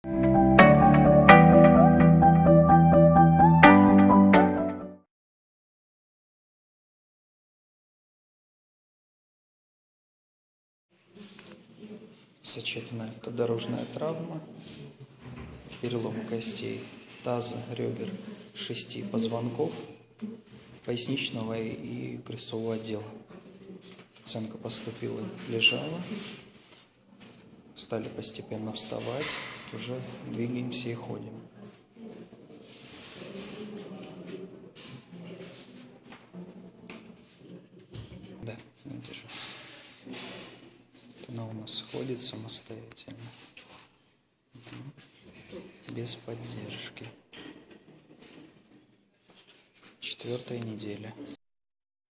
Реабилитация | Успех | Травма позвоночника | Spinal fracture
Реабилитацию в виде лечебной гимнастике при переломе тел позвонков любого отдела позвоночника (шейного, грудного или поясничного) начинают буквально с первых дней после получения травмы и оказания профессиональной помощи в специальном медицинском учреждении.